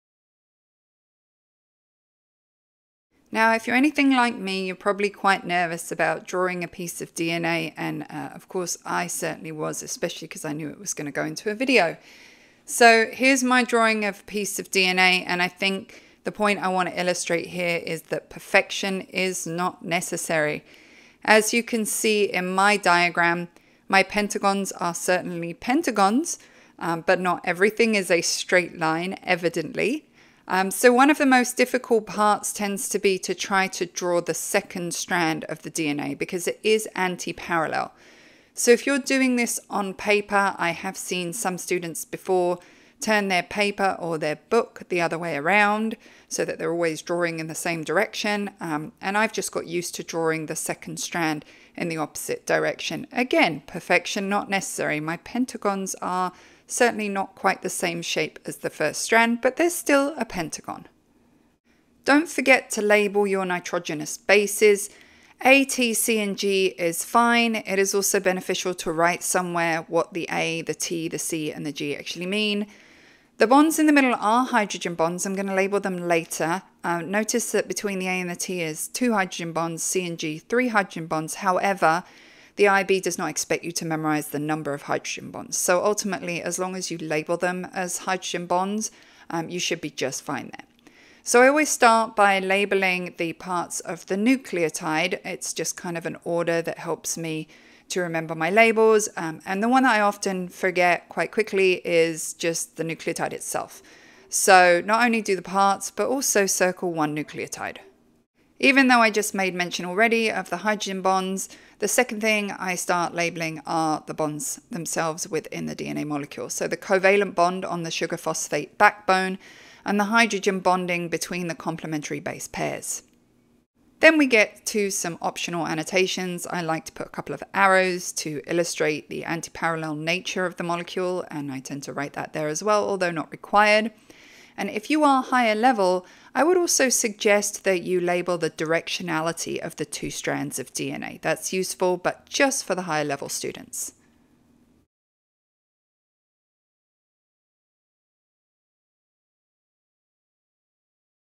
A1.2.6 Drawing the structure of DNA
This video addresses standard A1.2.6 in the new IB Biology syllabus with first exams 2025.